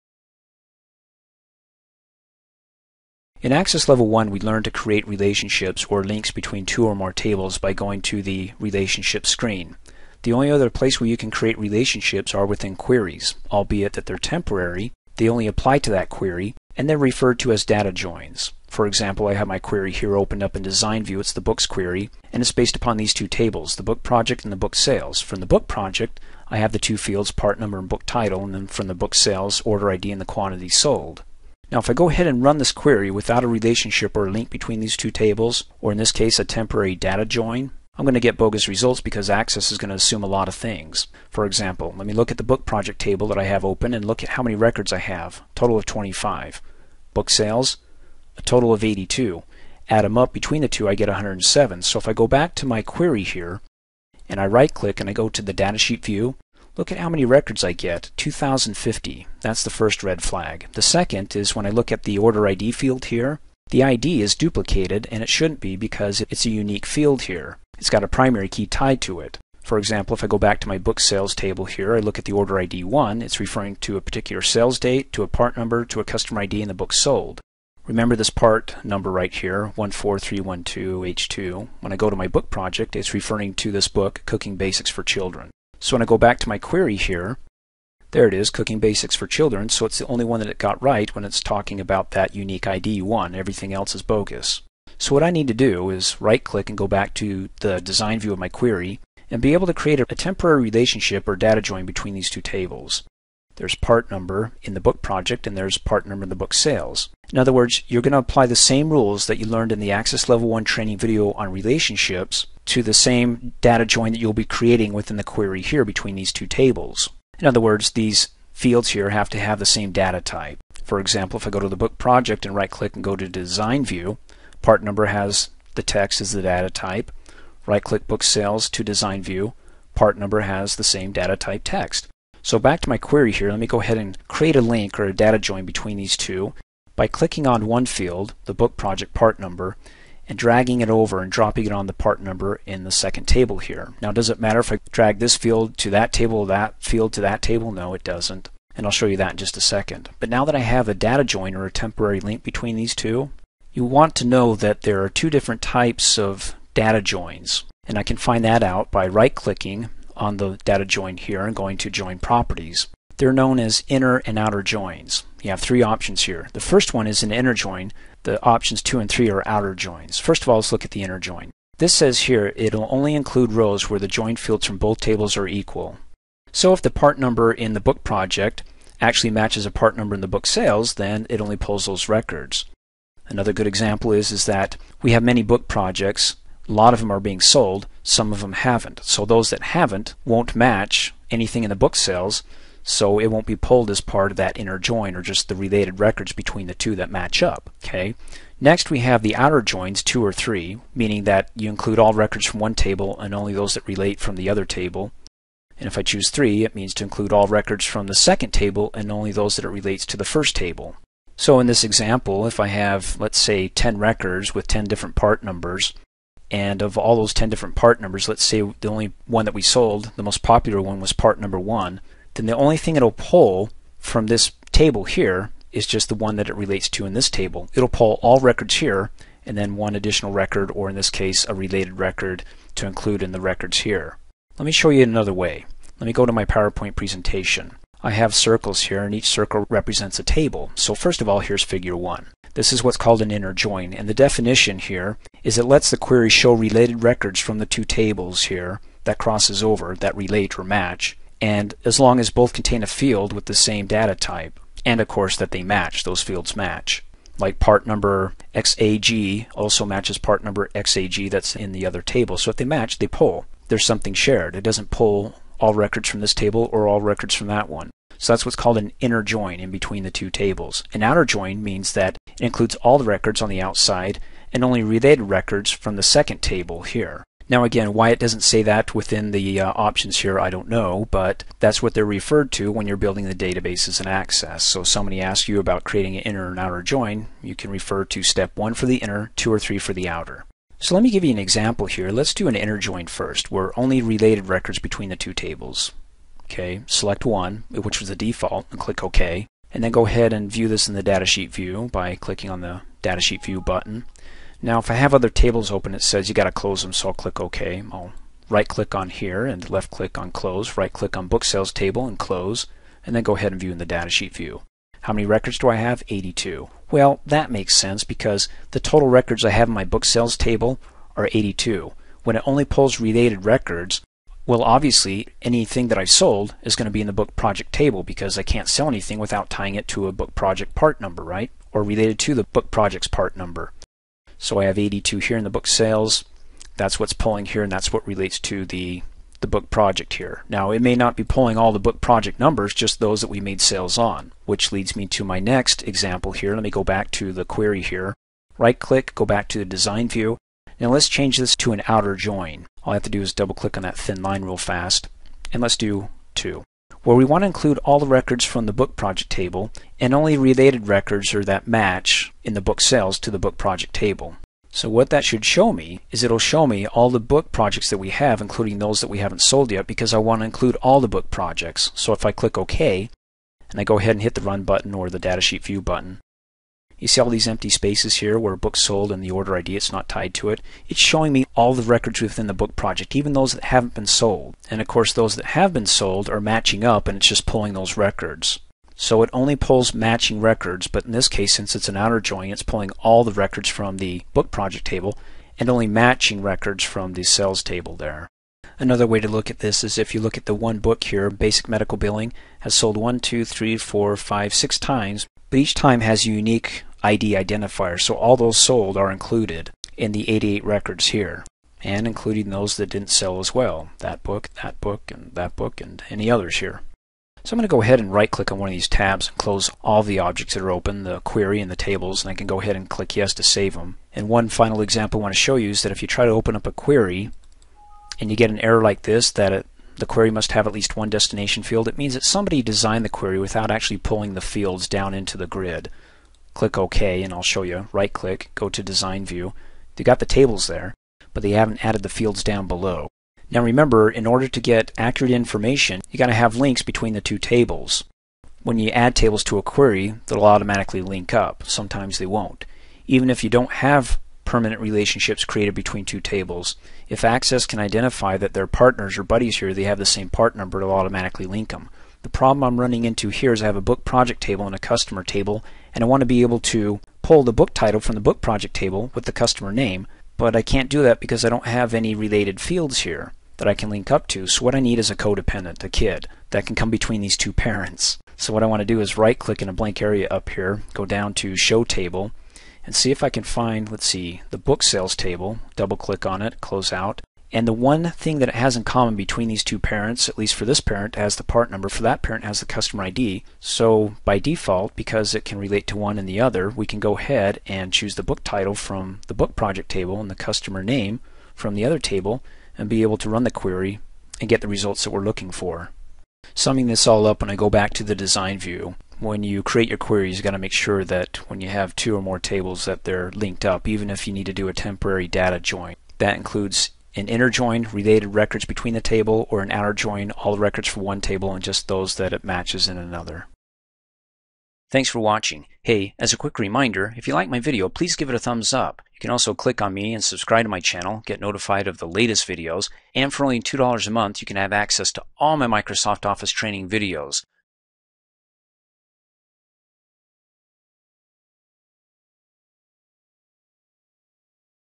Access 2007: Inner And Outer Query Data Joins
Access 2007 tutorial on query inner and outer data joins that allow you to create a temporary relationship between tables.